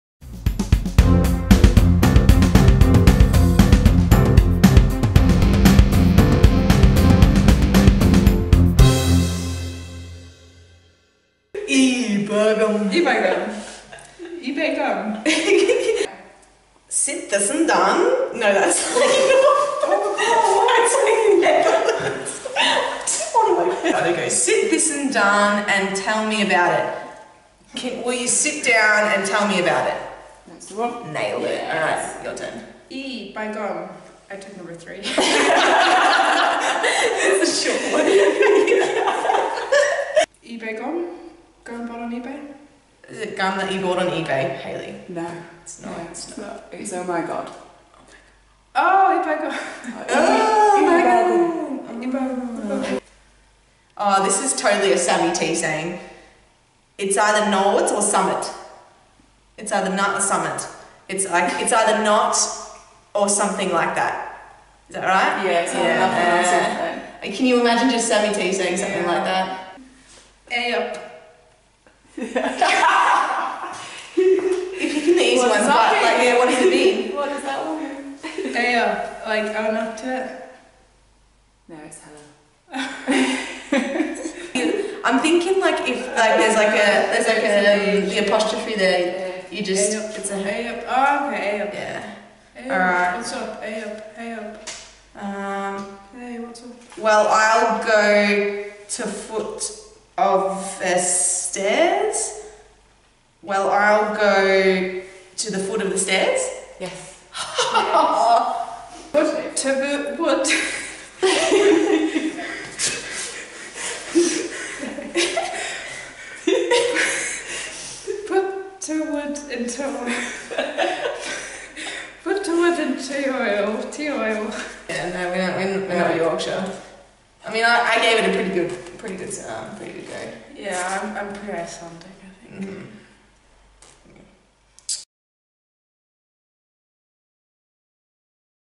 ACCENTS: Katrin & Alex Attempt Yorkshire
We challenged Doncaster Rovers Belles duo Alexandra Gummer and Katrin Omarsdottir - from Australia and Iceland respectively - to speak some proper Yorkshire...
'Ere's t'results!